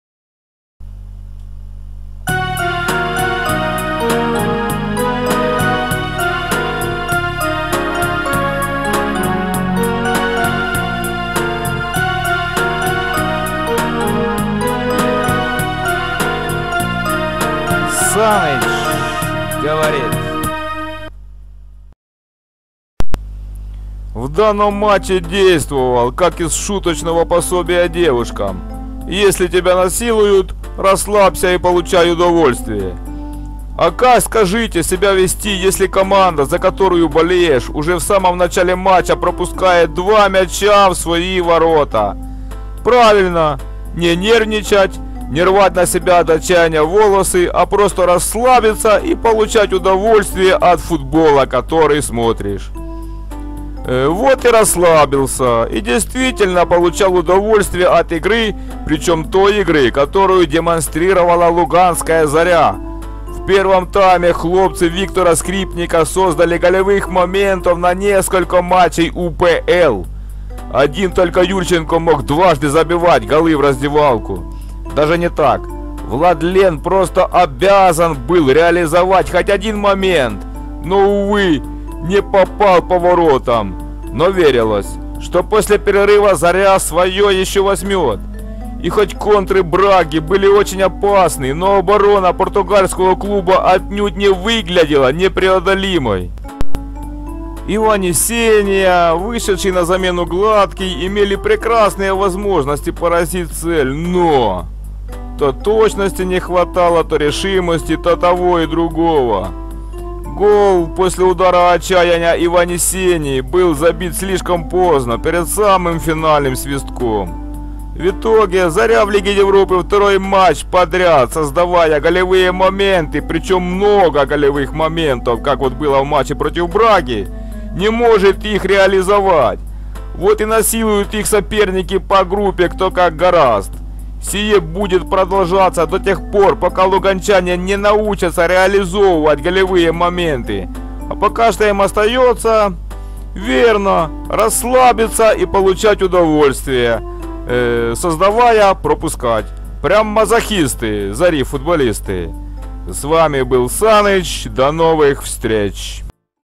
Заря – Брага 1-2. Мазохисты Зари
Лига Европы 2020-21. Заря – Брага 1-2. Мазохисты Зари – создают моменты, не забивают и пропускают, действуют по принципу – насилуют, расслабься и получи удовольствие.

ФутболСан